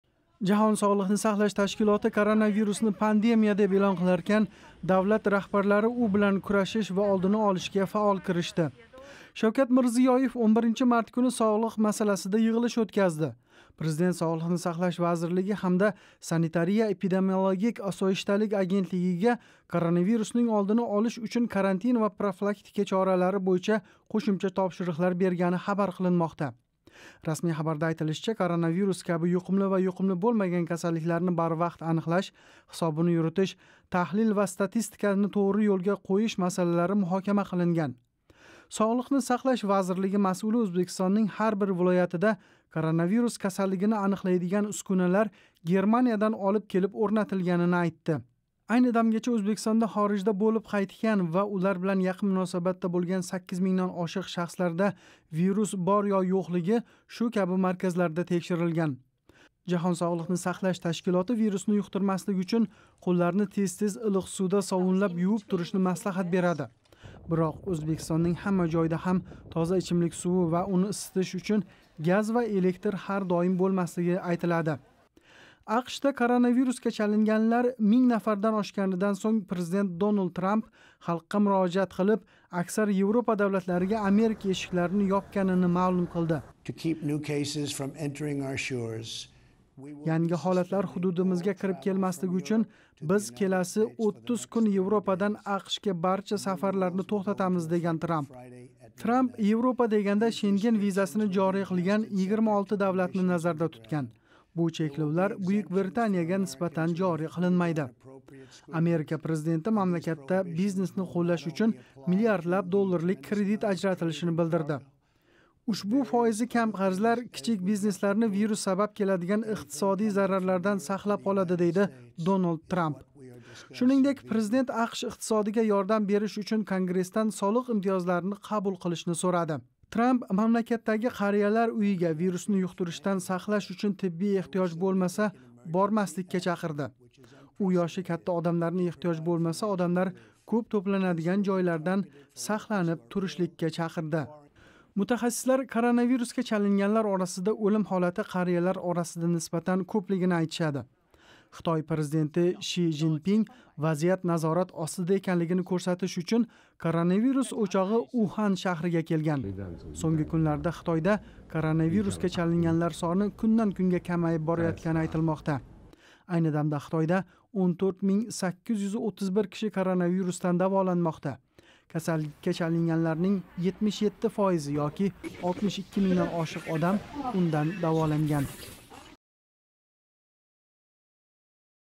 Коронавирус ва Президентлар: Трампдан Мирзиёевгача - Ўзбекистон, Америка, Хитой
Жаҳон соғлиқни сақлаш ташкилоти коронавирусни пандемия деб эълон қиларкан, давлат раҳбарлари у билан курашиш ва олдини олишга фаол киришди. Шавкат Мирзиёев 11 март куни соғлиқ масаласида йиғилиш ўтказди. Трамп Американи Европа давлатларига ёпди.